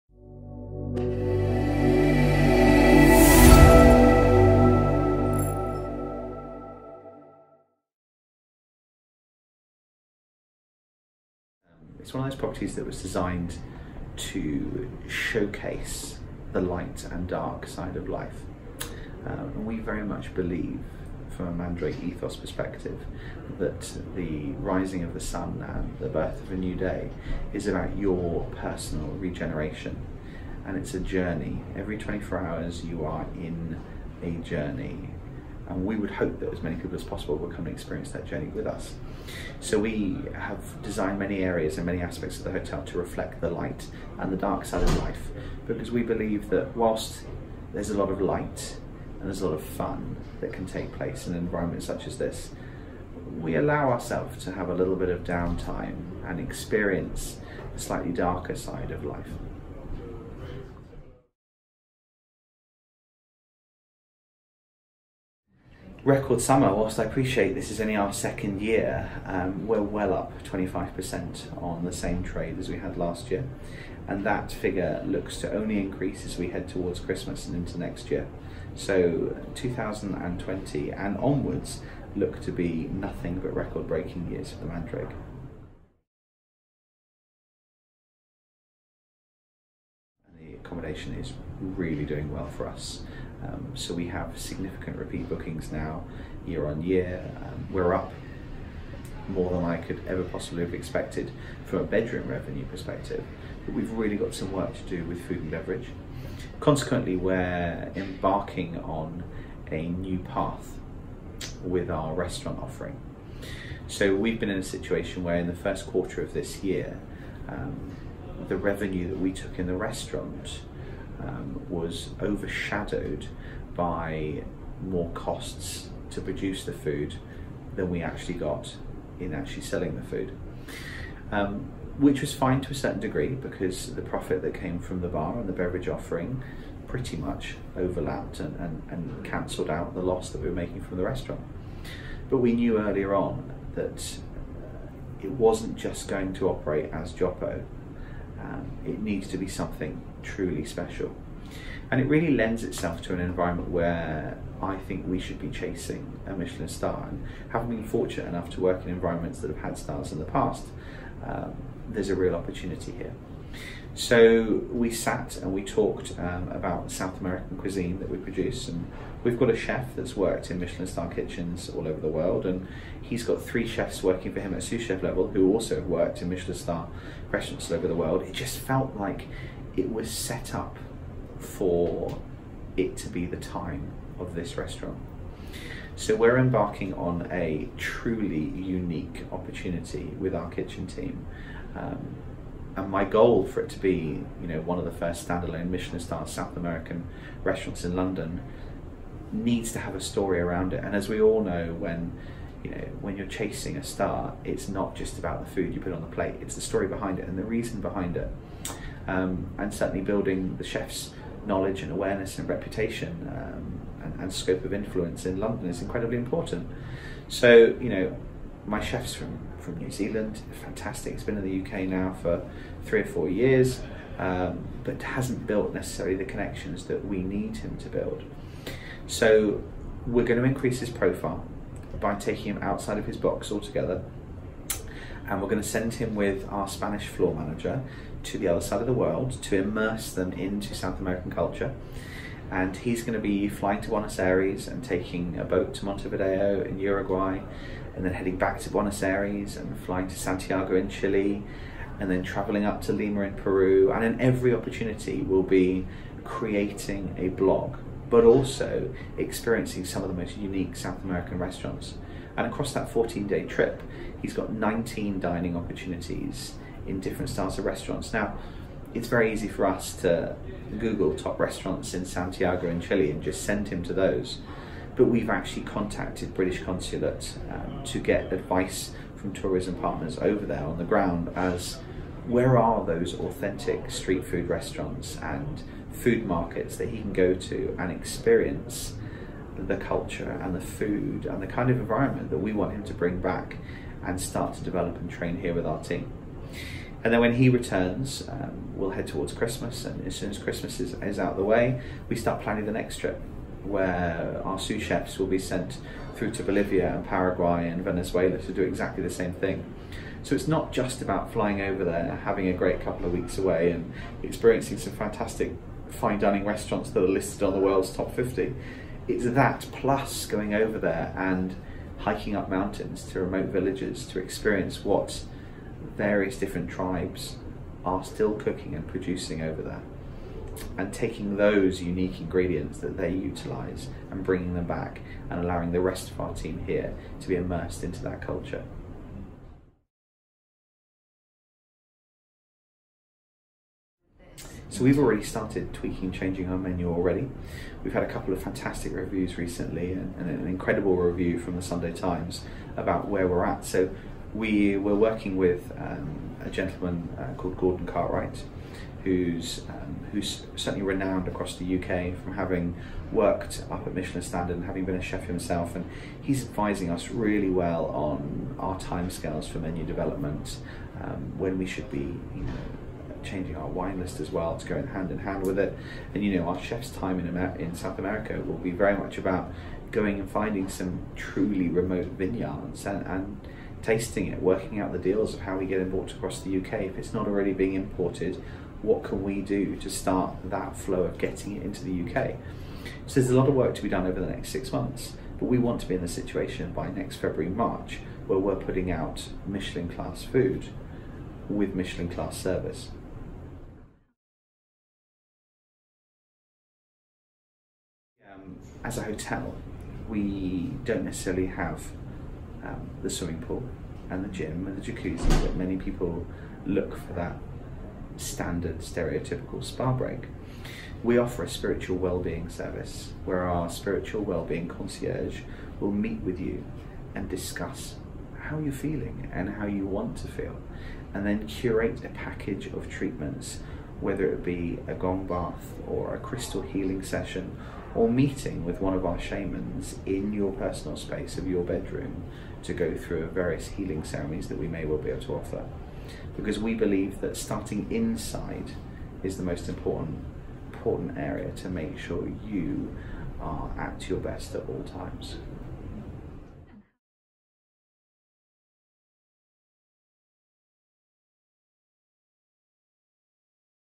Simon Drake on The Mandrake's Michelin-star goal
BHN spoke to Simon Drake, general manager at The Mandrake, about the message behind the hotel and its food and beverage strategy.